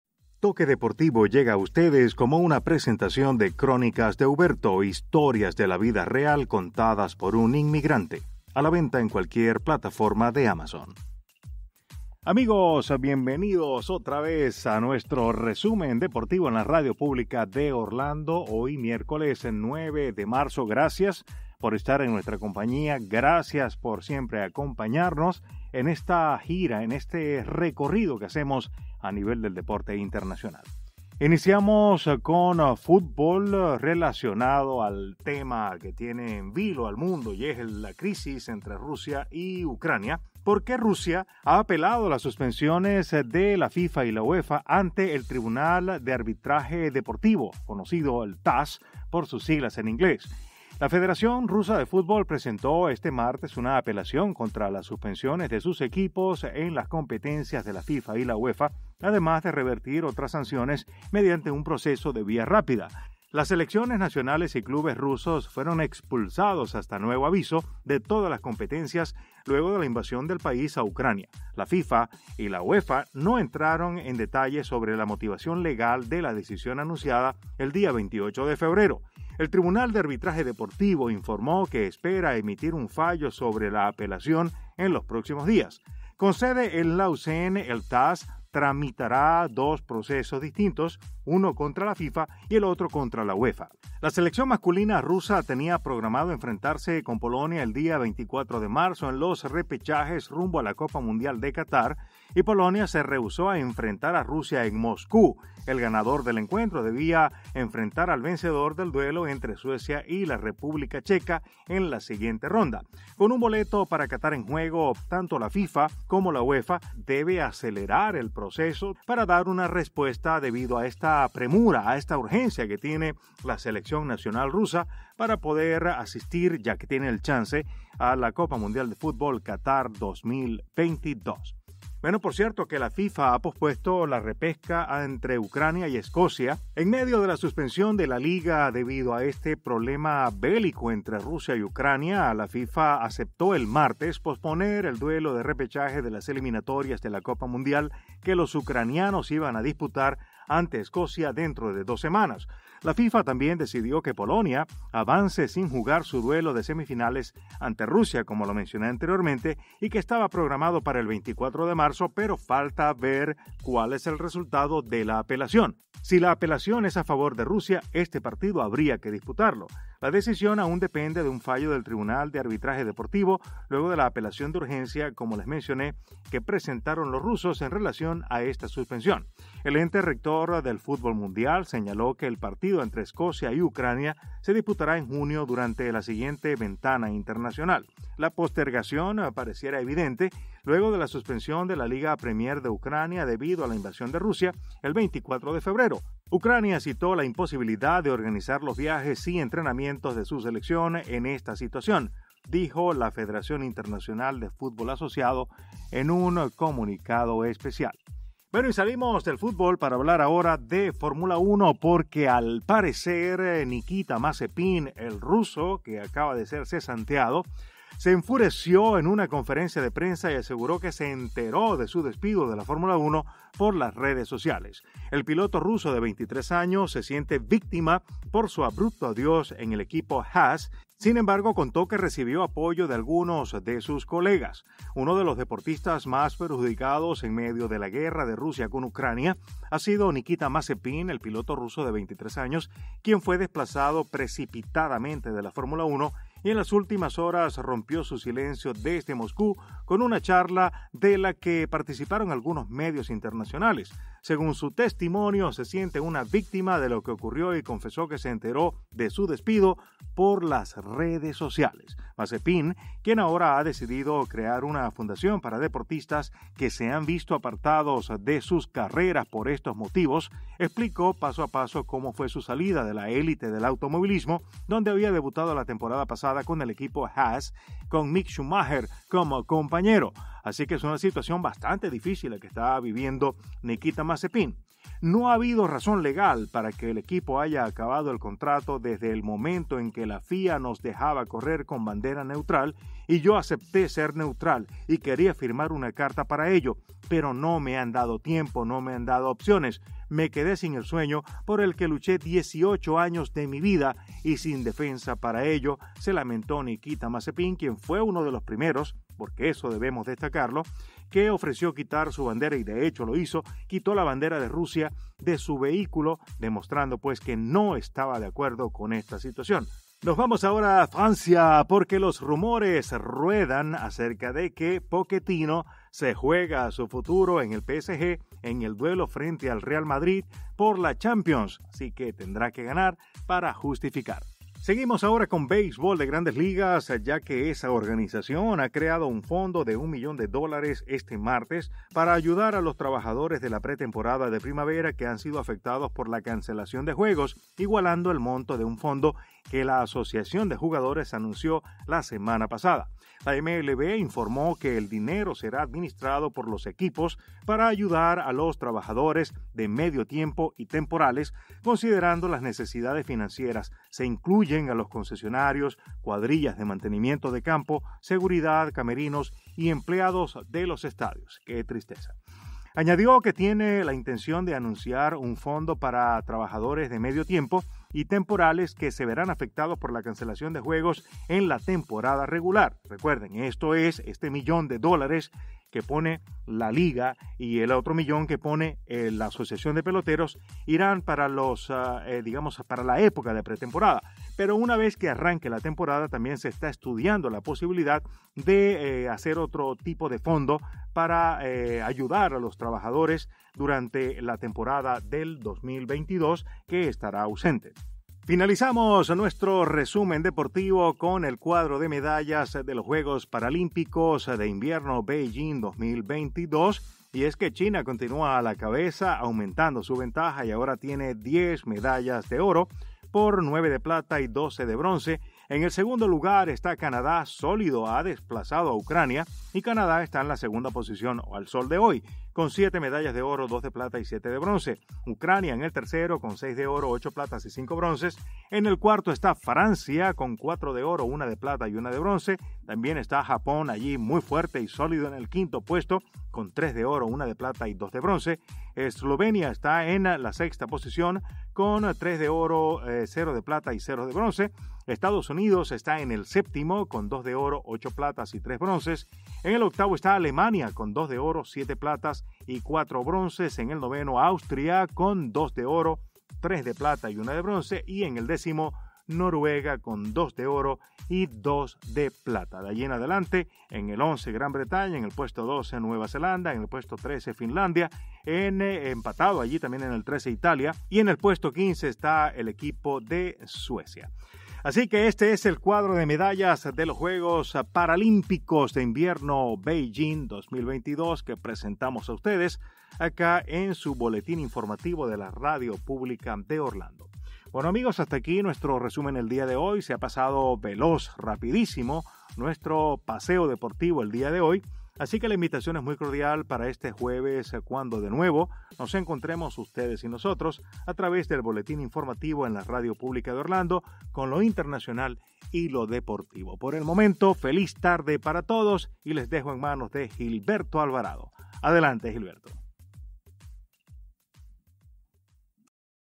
Rusia apeló ante el TAS la suspensión por parte fe la Fifa y la Uefa 9MAR
Rusia apelo ante el TAS la suspensión por parte fe la Fifa y la Uefa / La Fifa pospone el choque entre Ucrania y Escocia / Piloto ruso de F1 botado por Haas dio una Rueda de Prensa donde descargo su furia e impotencia por su despido injustificado / Las Grandes Ligas crean fondo de 1 millones de dólares al igual que la Asociacion de jugadores / China amplia ventaja en los Paralímpicos de invierno en Beijing2022